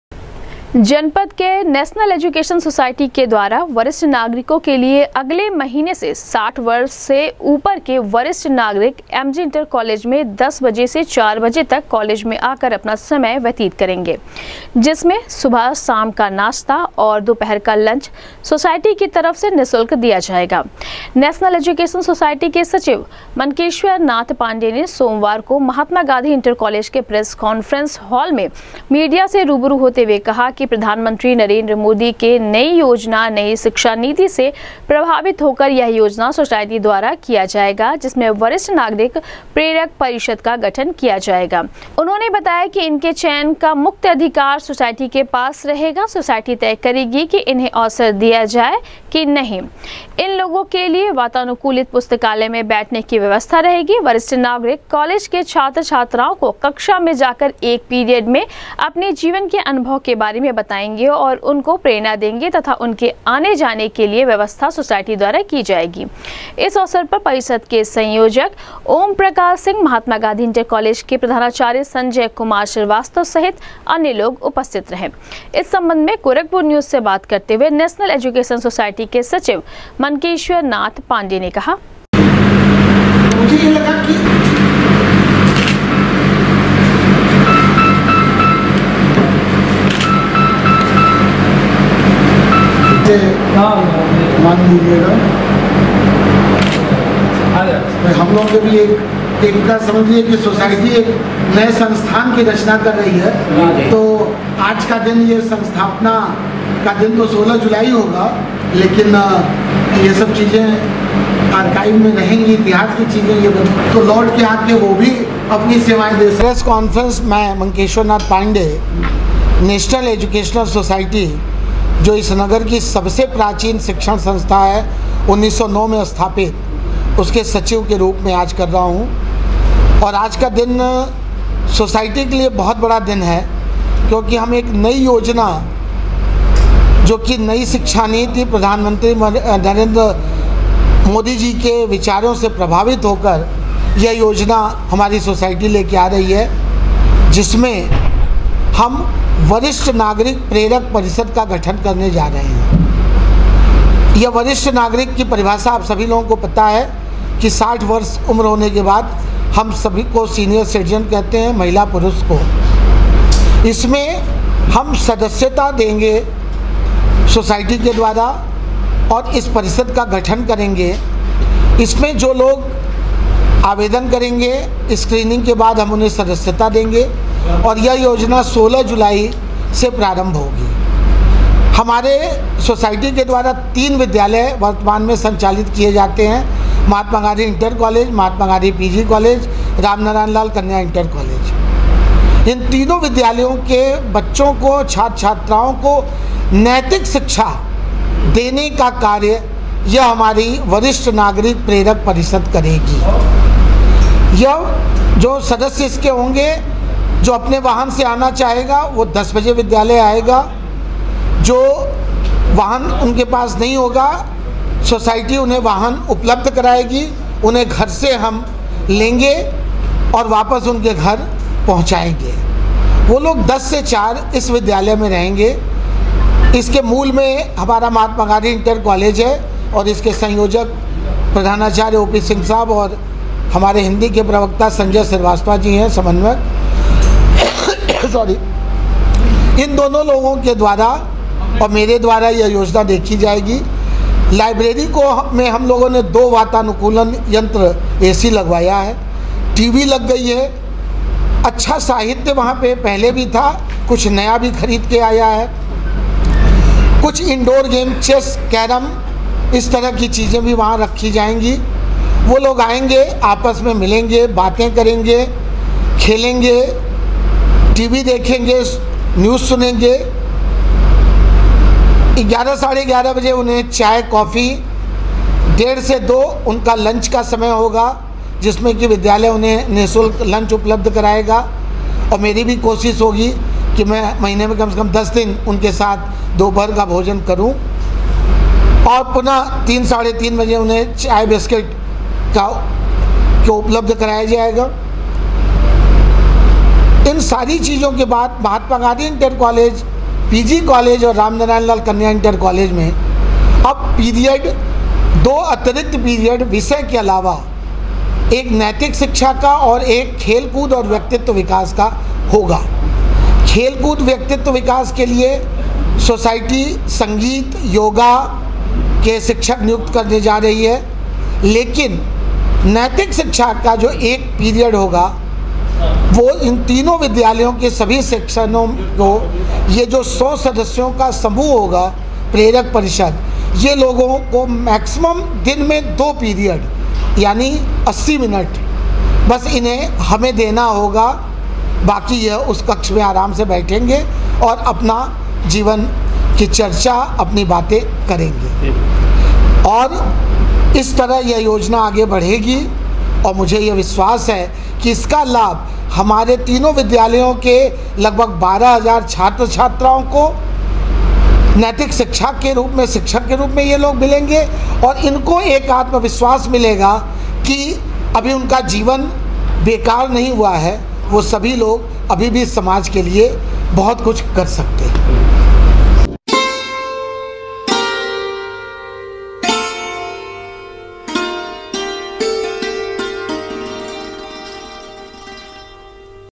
एमजी इंटर कॉलेज की संचालक मातृसंस्था नेशनल एजुकेशन सोसाइटी ने वरिष्ठ नागरिक प्रेरक परिषद का
एमजी इंटर कॉलेज की संचालक मातृसंस्था नेशनल एजुकेशन सोसाइटी ने वरिष्ठ नागरिक प्रेरक परिषद का गठन करने का किया ऐलान,कहा, वरिष्ठ नागरिक कॉलेज केे छात्र छात्राओं को उनकी कक्षाओं में जाकर अपने जीवन के अनुभव को साझा कर देंगे छात्र छात्राओं को प्रेरणा ।।